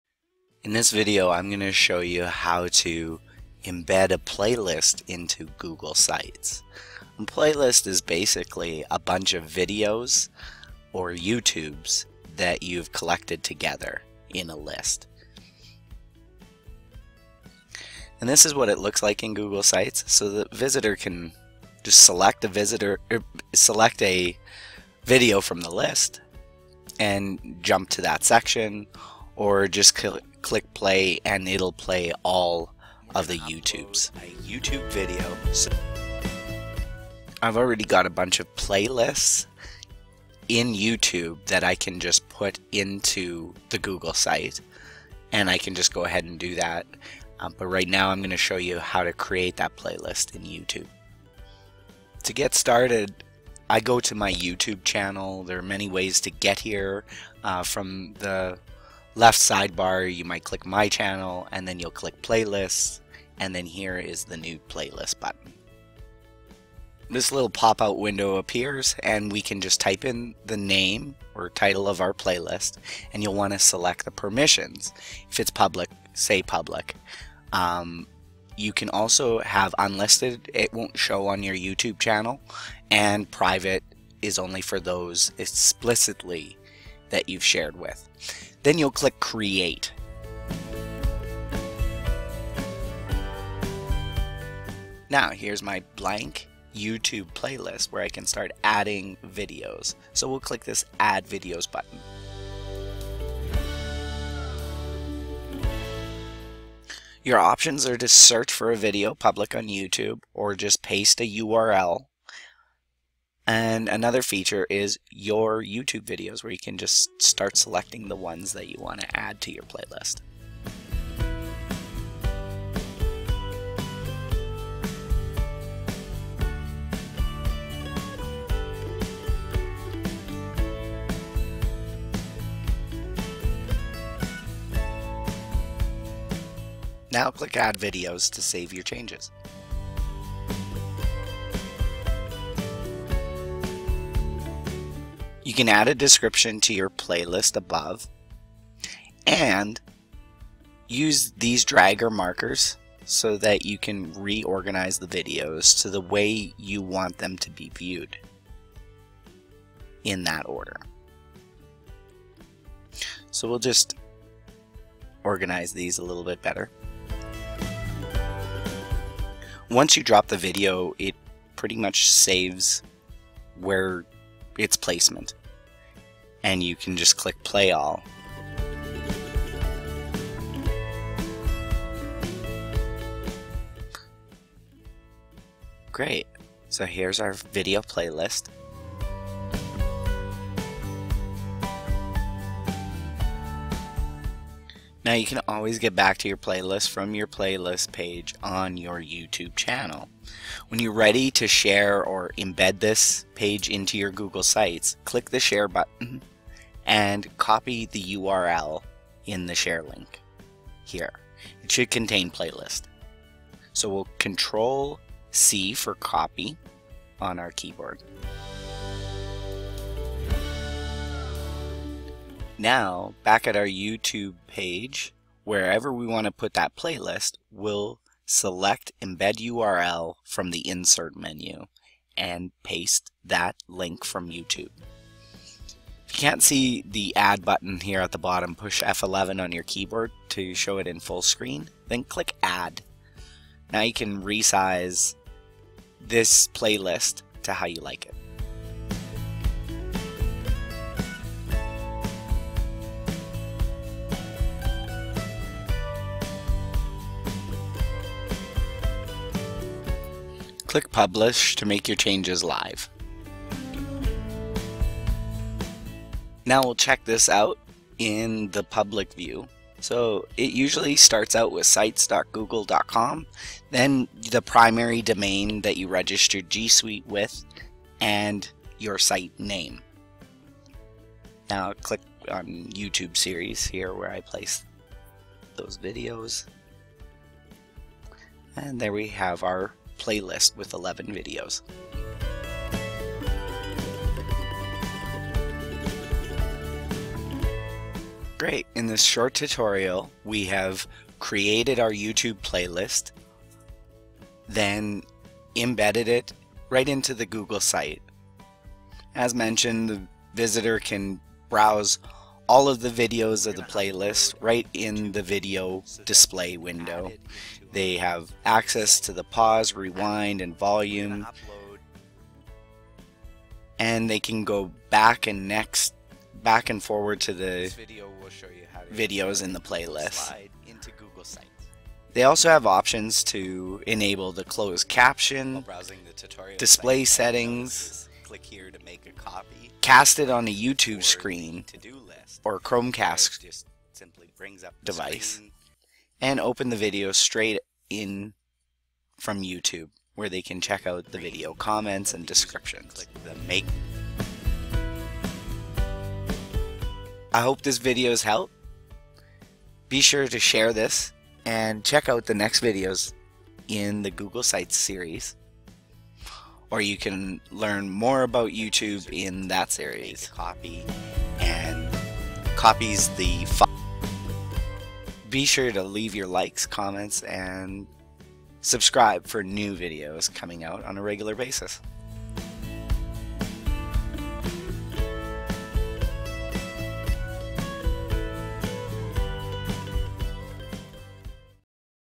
Create and Add a YouTube Playlist to the New Google Sites
In this video I will show how to make a YouTube Playlist and embed it on to the New Google Sites.

From the Playlist Menu on your YouTube Channel
Copy the Playlist URL
In the New Google Site go to the page you want to insert the playlist.  Click Insert, Click Embed and past the YouTube URL